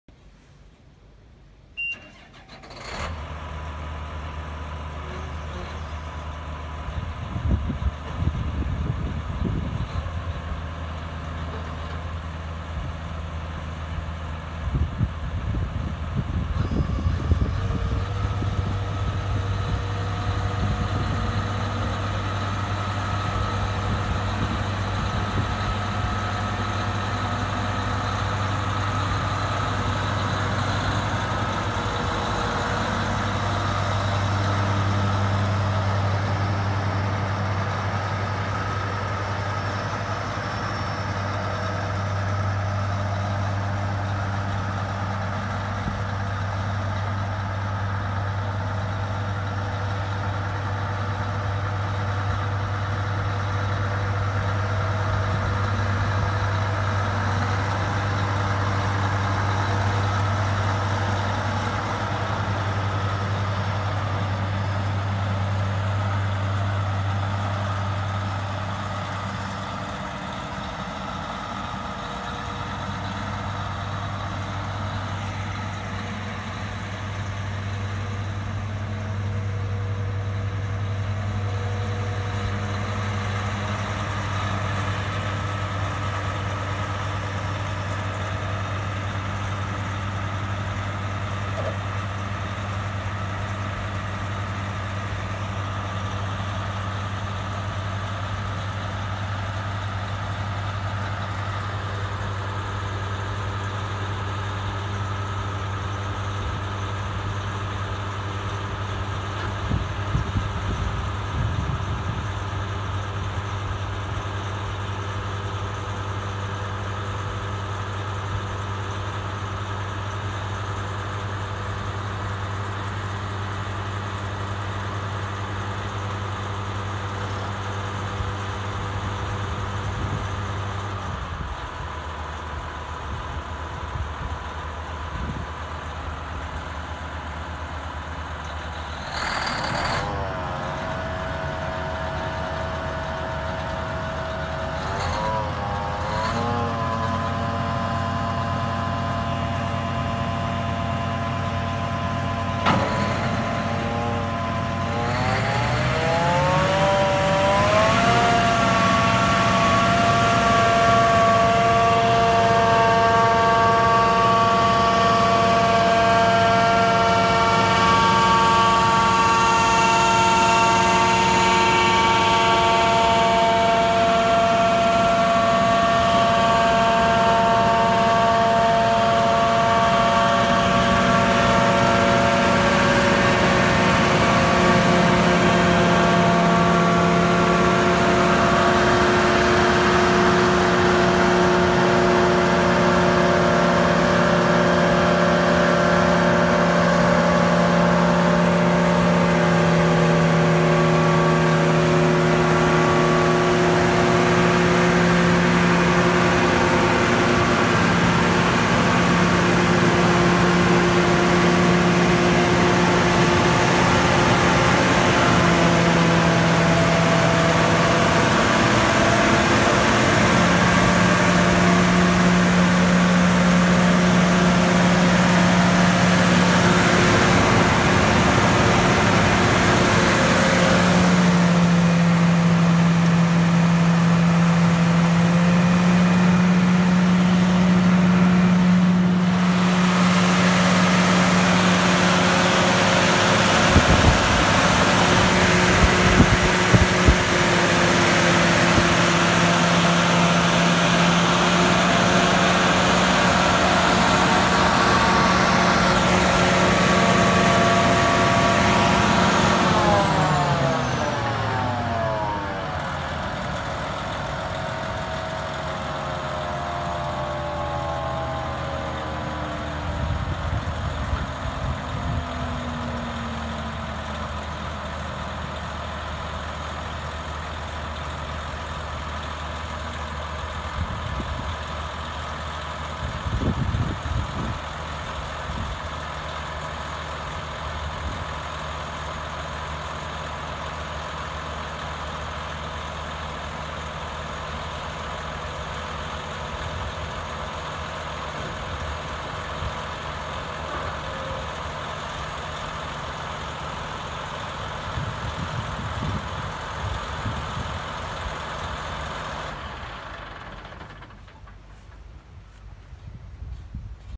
Bigiron Online Auctions 2008 7450 ProDrive Chopper
Jim Herzog Retirement Auction
Selling December 20th 2017
Auctions Every Wednesday at 10:00AM